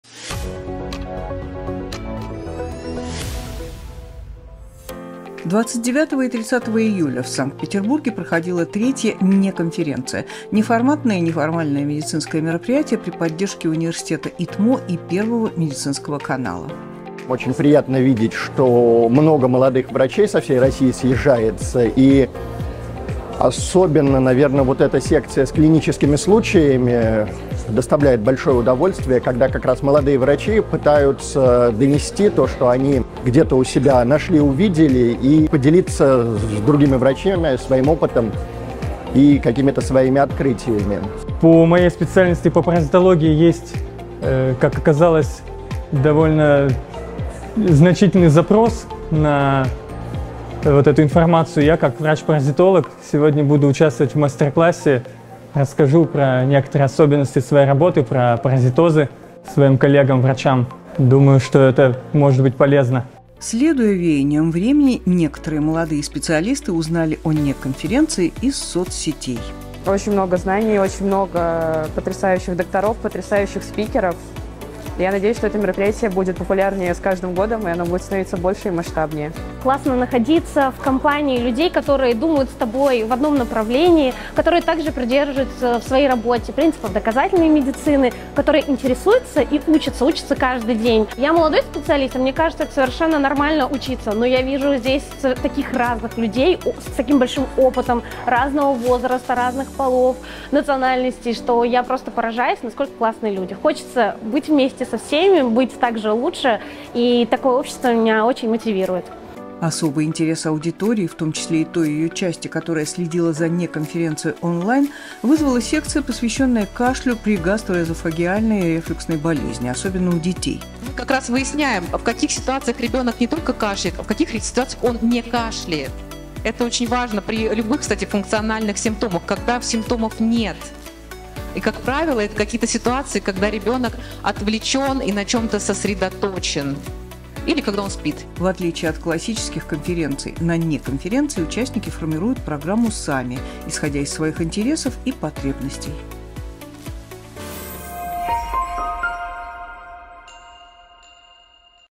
(НЕ)КОНФЕРЕНЦИЯ 2023 // Репортаж 1MEDTV
29 и 30 июля 2023 года в Санкт-Петербурге проходила третья (НЕ)конференция – неформатное и неформальное медицинское мероприятие образовательного проекта Doc2Doc при поддержке Университета ИТМО и Первого медицинского канала.

В отличие от классических конференций, на не-конференции участники формируют программу сами, исходя из своих интересов и потребностей. В программе мероприятия были как лекционные сессии, так и мастер-классы по различным нозологиям.

Смотрите наш репортаж с мероприятия!